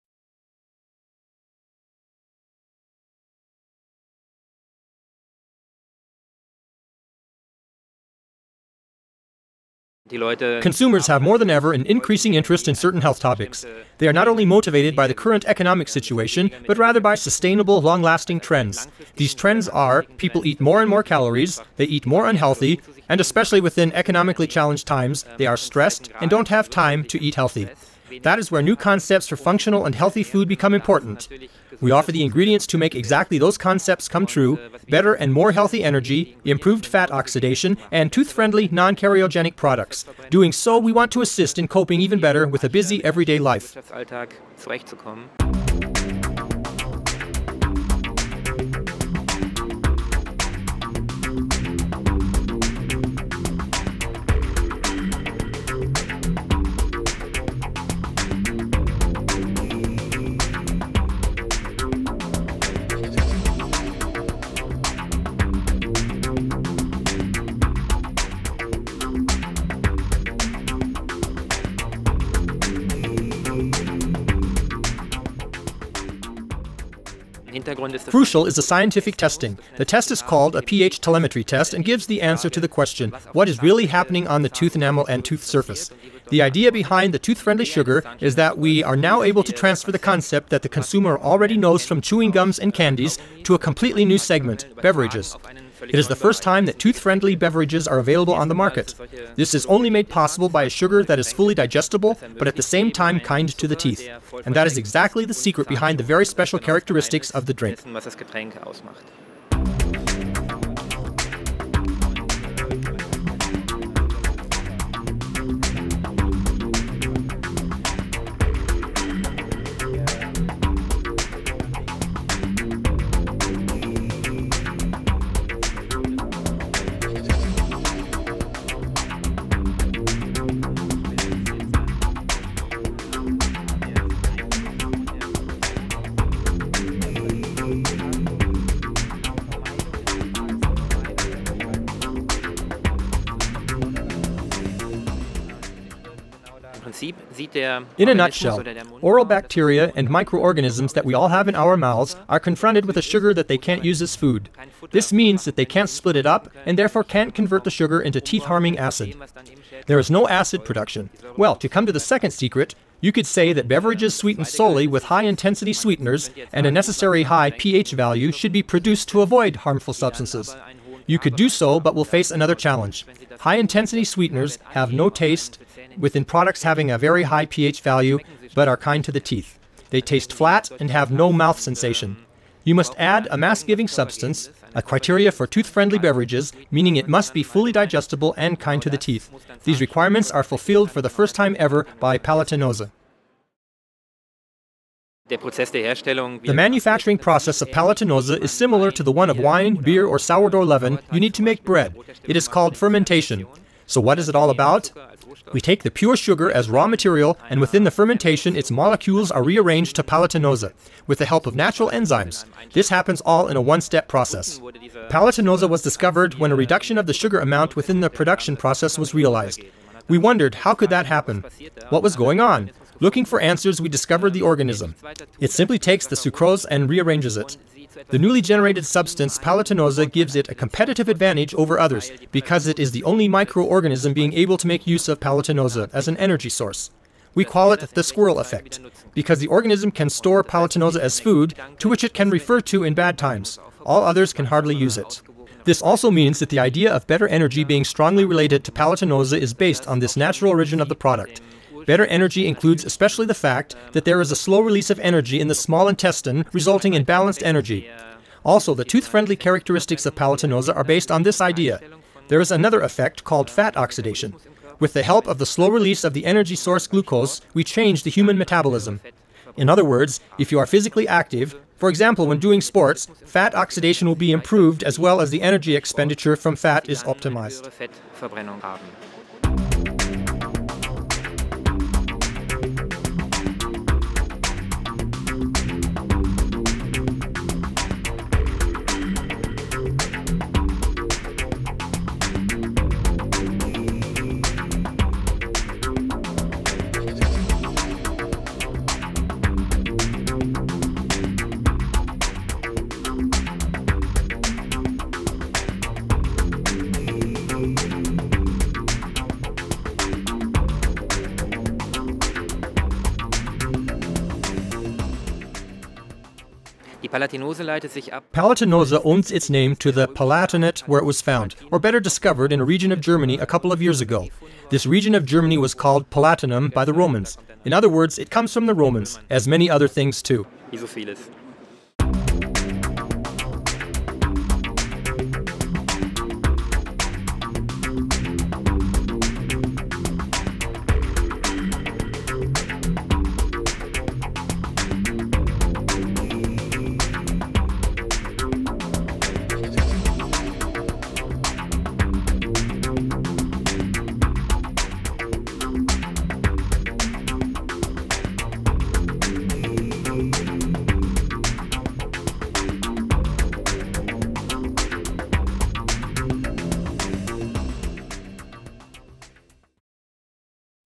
The first toothfriendly sugar
Interview with Dr. Stephan Hausmanns about Palatinose(TM) the first non-cariogenic and fully digestible sugar.